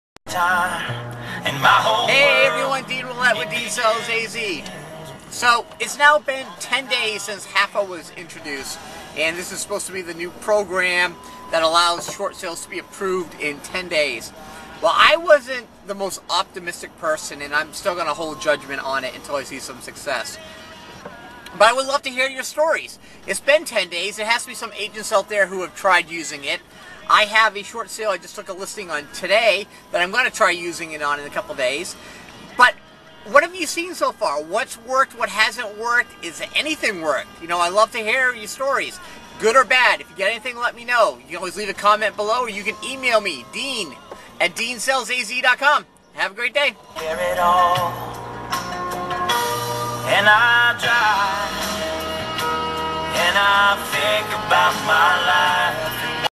111 HAFA: Is it working?
I have talked about HAFA a lot It has been around for 10 days is it working or not? Yea I know it is early, but let me hear your stories.